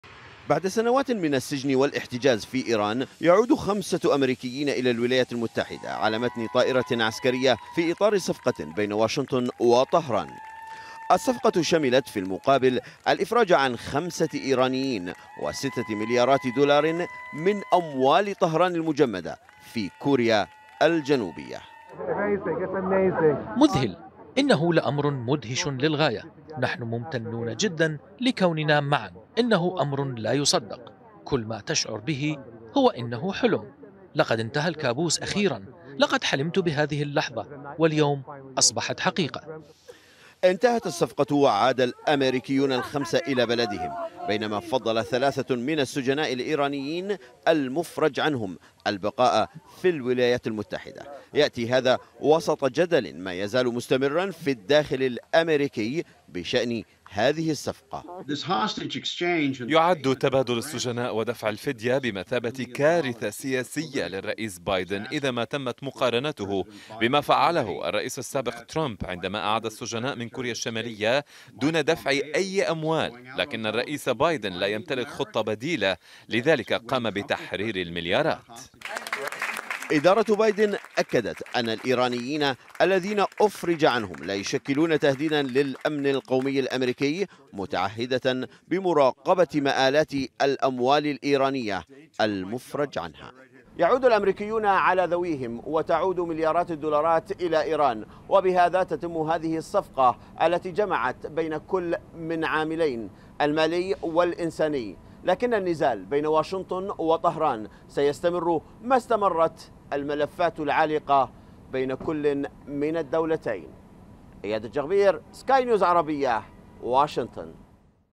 وصول 5 أميركيين إلى الولايات المتحدة بعد الإفراج عنهم من إيران
وصل الأميركيون الخمسة الذين أفرجت عنهم إيران إلى الولايات المتحدة ضمن صفقة تبادل السجناء بين واشنطن وطهران.
هذا وتعهد البيت الأبيض بمراقبة مآلات الأموال الإيرانية المفرج عنها ضمن صفقة التبادل..